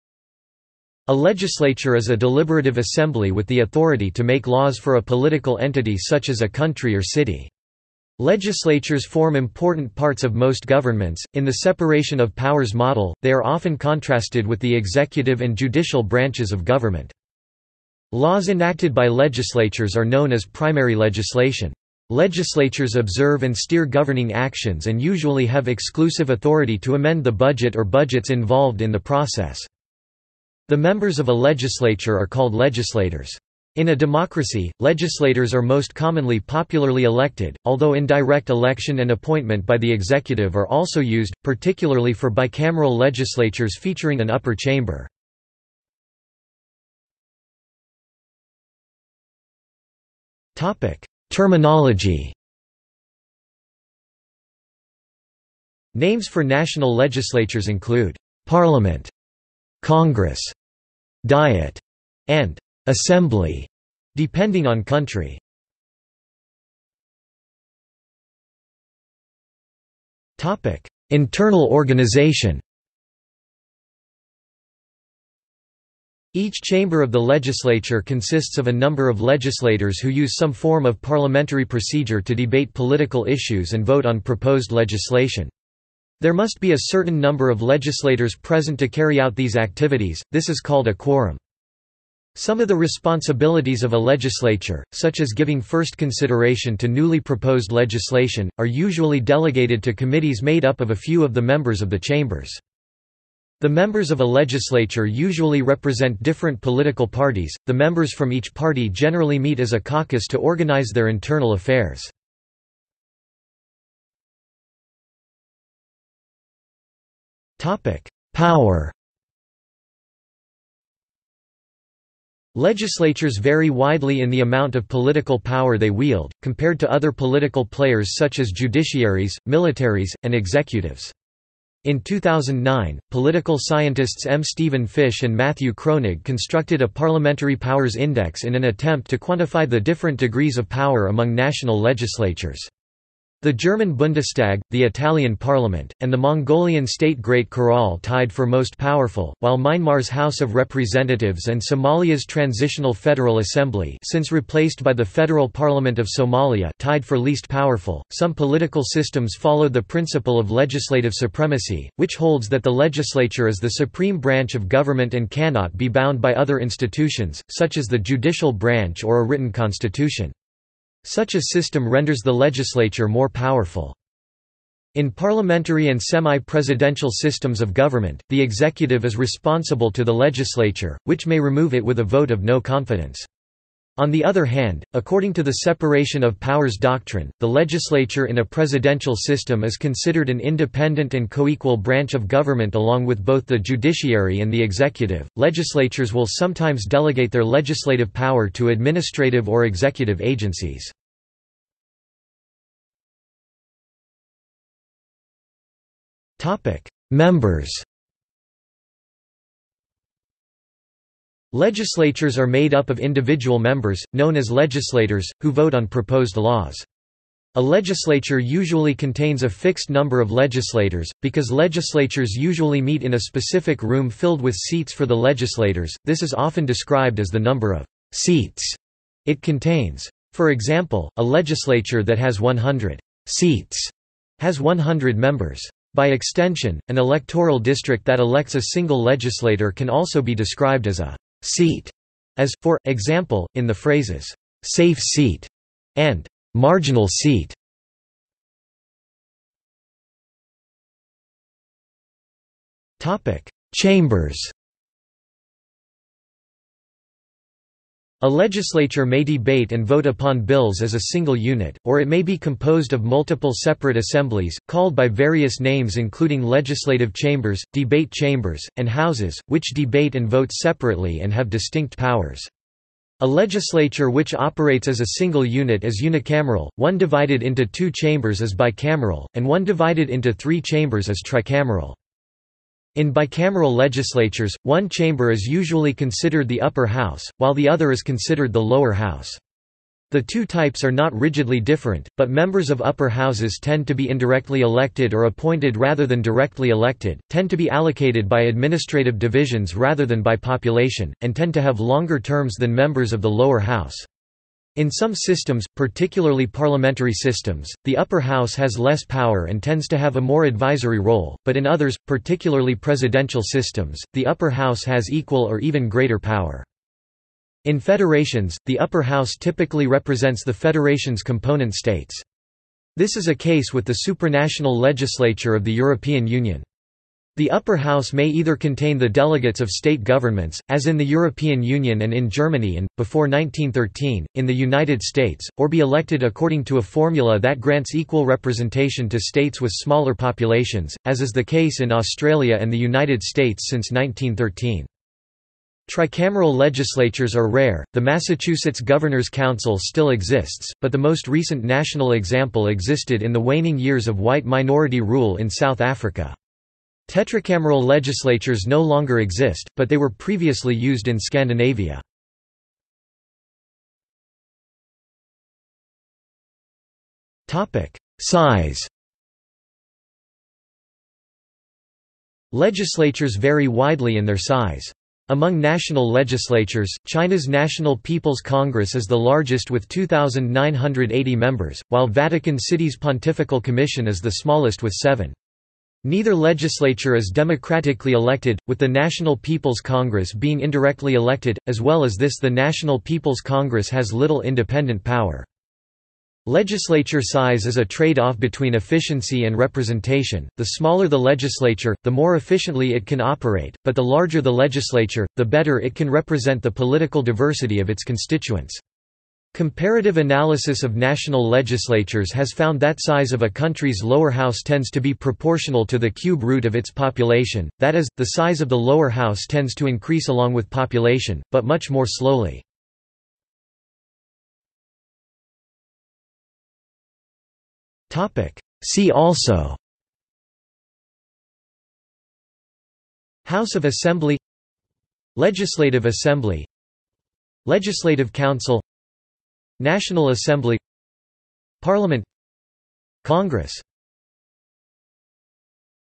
Legislature | Wikipedia audio article
This is an audio version of the Wikipedia Article:
Legislature

00:00:46 1 Terminology
00:01:06 2 Internal organization
00:01:54 3 Power
00:03:22 4 Members
00:04:09 5 Chambers
00:06:03 6 Size
00:07:04 7 See also

- Socrates

SUMMARY
A legislature is a deliberative assembly with the authority to make laws for a political entity such as a country or city. Legislatures form important parts of most governments; in the separation of powers model, they are often contrasted with the executive and judicial branches of government.
Laws enacted by legislatures are known as primary legislation. Legislatures observe and steer governing actions and usually have exclusive authority to amend the budget or budgets involved in the process.
The members of a legislature are called legislators. In a democracy, legislators are most commonly popularly elected, although indirect election and appointment by the executive are also used, particularly for bicameral legislatures featuring an upper chamber.